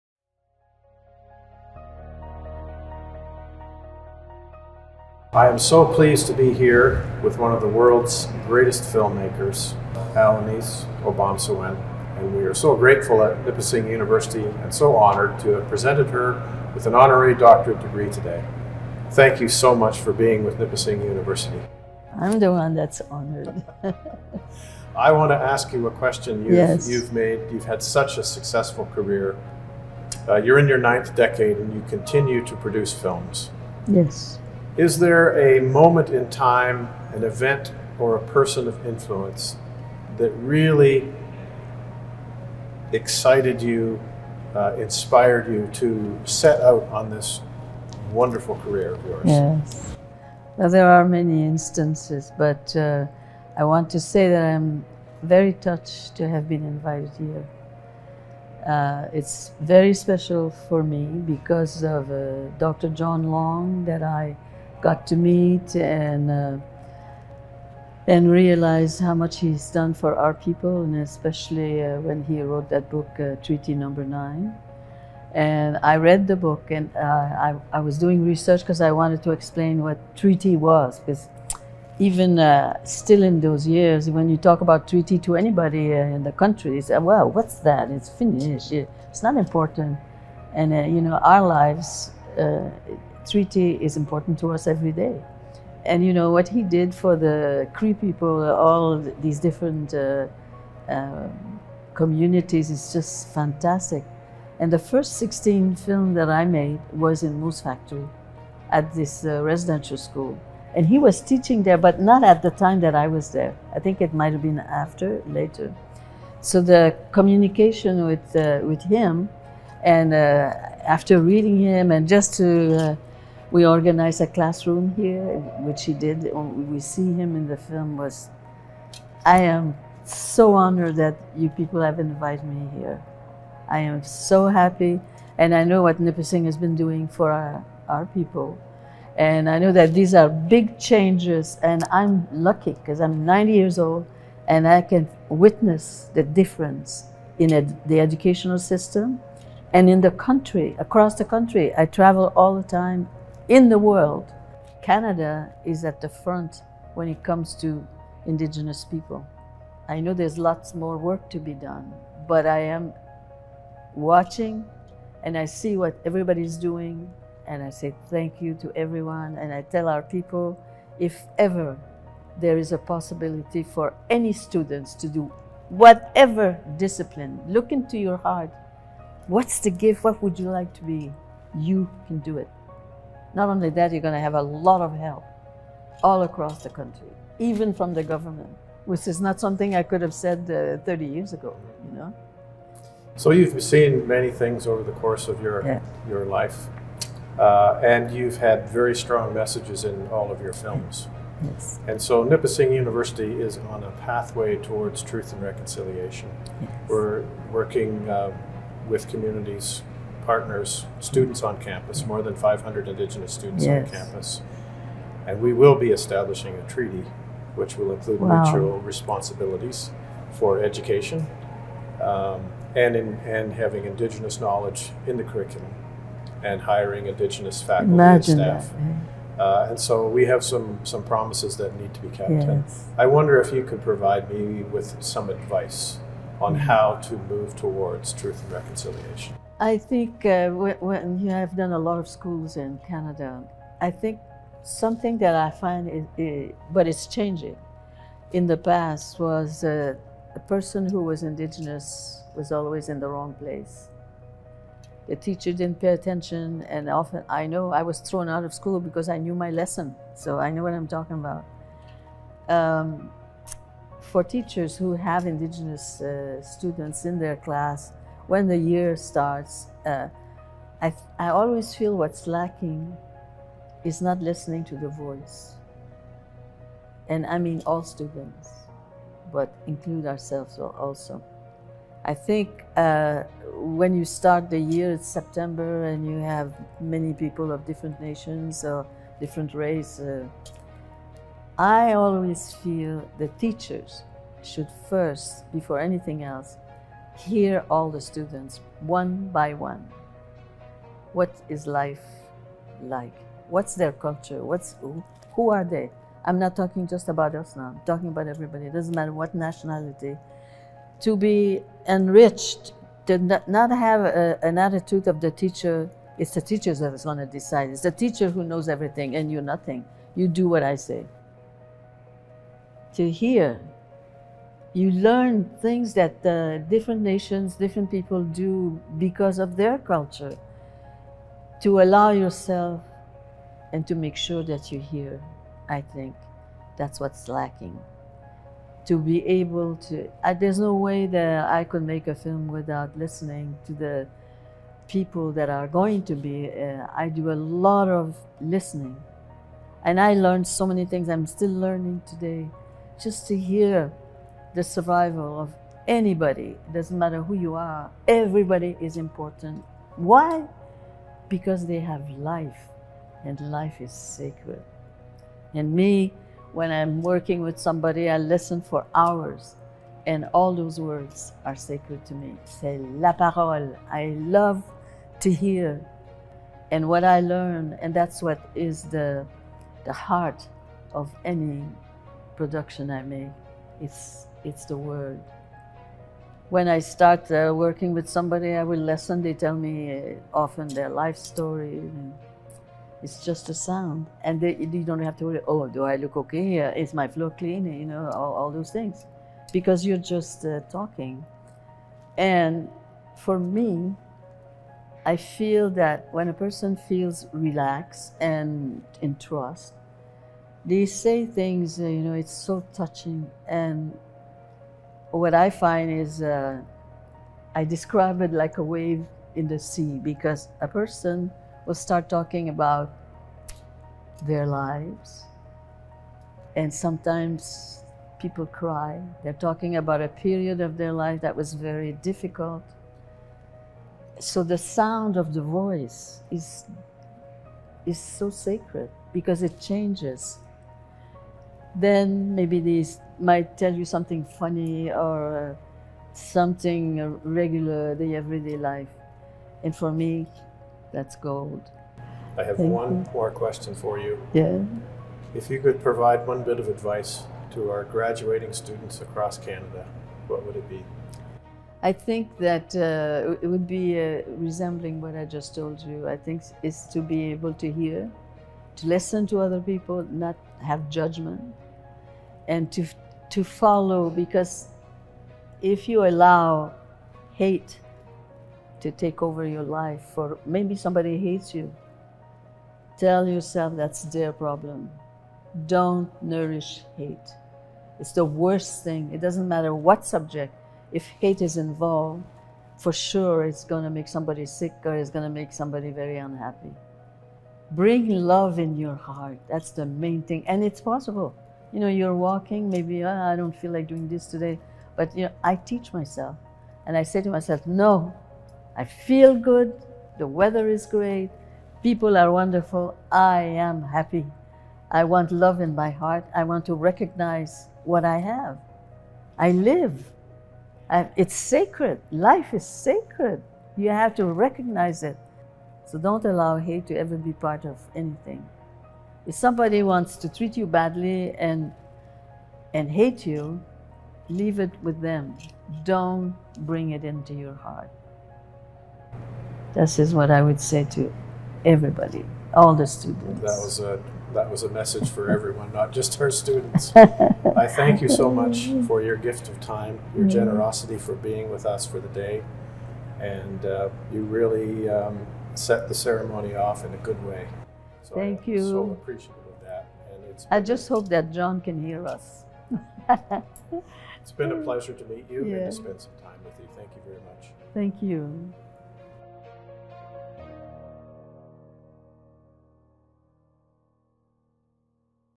A Conversation with Alanis Obomsawin, Honorary Degree Recipient 2023
Alanis Obomsawin, one of Canada’s most distinguished filmmakers, was presented with an honorary degree from Nipissing University for her excellence in filmmaking and commitment to Truth and Reconciliation through her storytelling. She sat down with President and Vice-Chancellor, Dr. Kevin Wamsley, to discuss the power listening has in achieving Truth and Reconciliation and a better understanding of each other as well as the importance of living without hate.